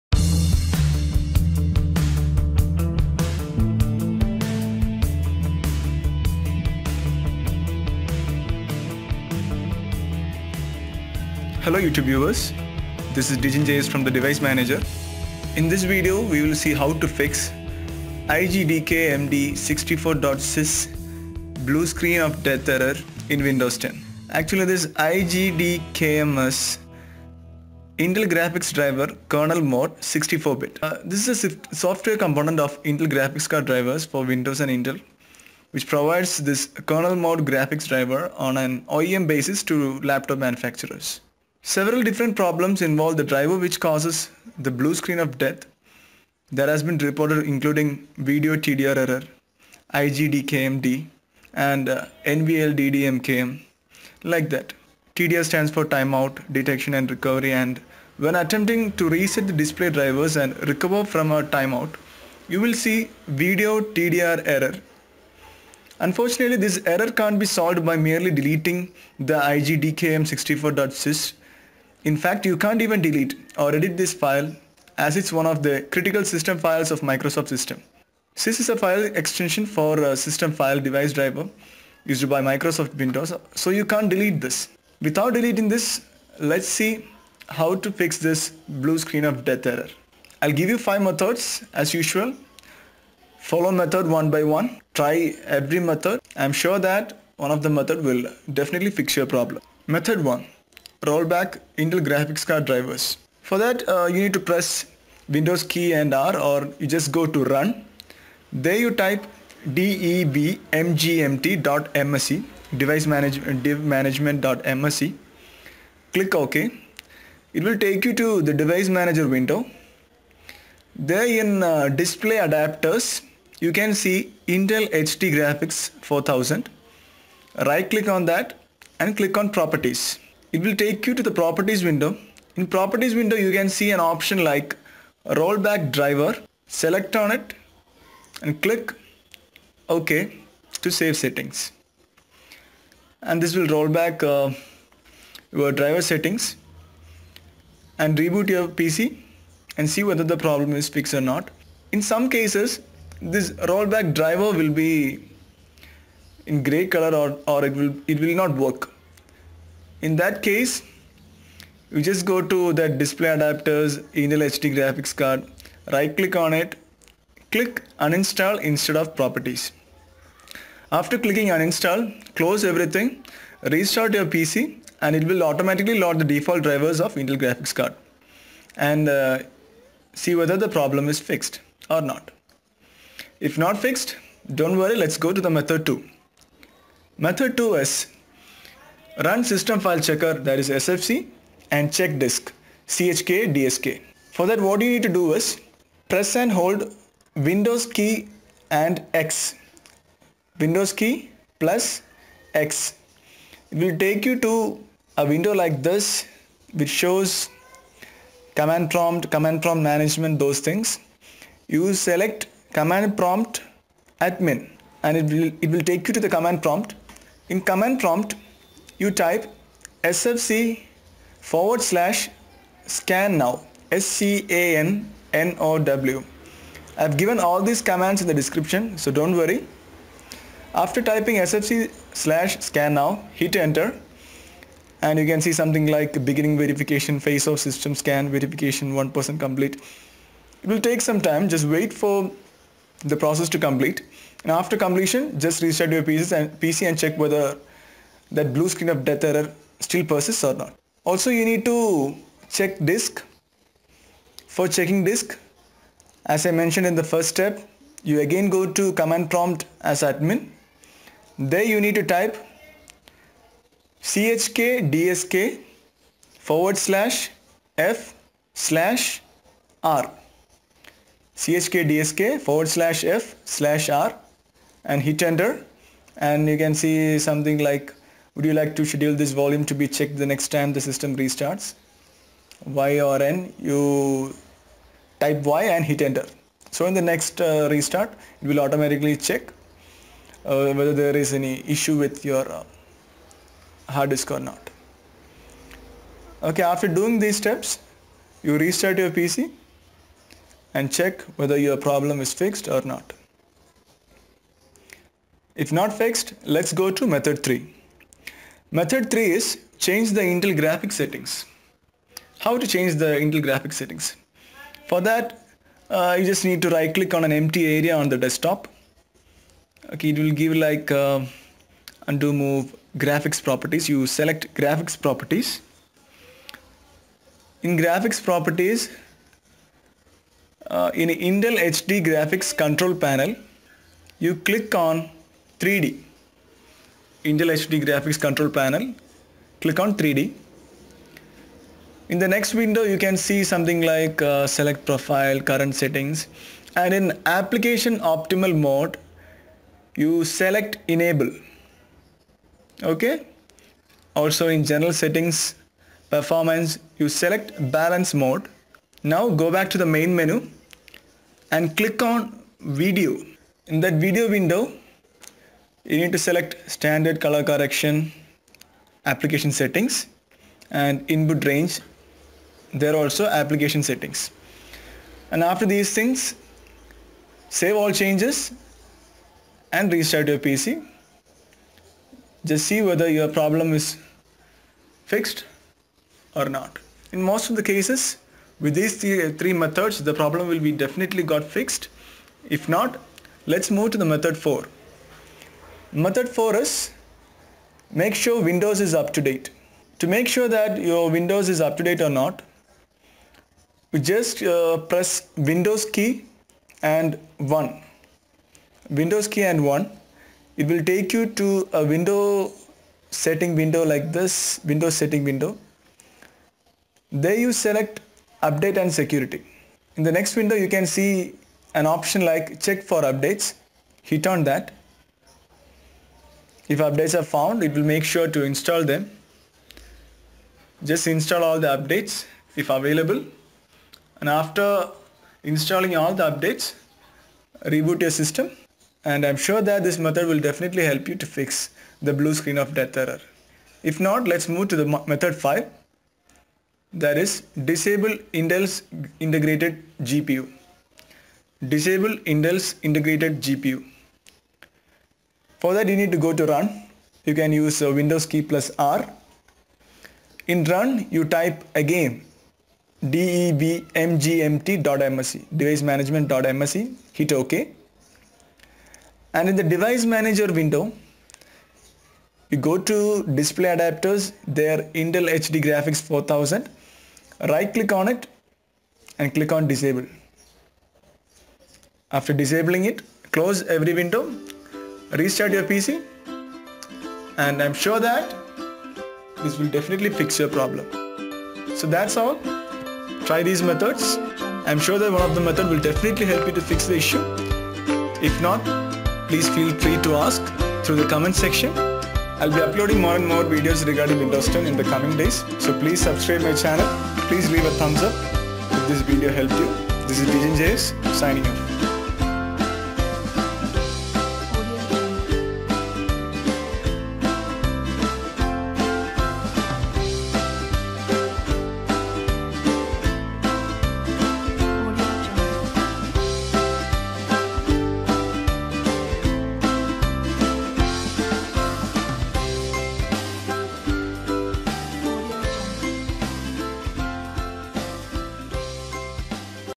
How to Fix Windows 10 Blue Screen WHEA UNCORRECTABLE ERROR 0x00000124
This video shows How to Fix Windows 10 Blue Screen  UNCORRECTABLE ERROR 0x00000124.igdkmd64.sys is a software component of Intel Graphic Card drivers for Windows and intel provides this kernel mode graphics driver on an OEM basis to laptop manufacturers. IGDKMd64 stands for Intel Graphics Driver Kernel Mode 64-bit. Several different problems involve this driver which causes the Blue Screen of Death  have been reported including VIDEO_TDR_ERROR, igdkmd64.sys, and nvlddmkm.sys.

Method 1: Roll back Intel Graphic Card Drivers

Method 2: Run System File Checker and Check Disk
Sfc /scannow
chkdsk/f/r

Method 3: Change the Intel graphics settings

Method 4: Make sure Windows is up to date

Method 5: Disable Intel’s integrated GPU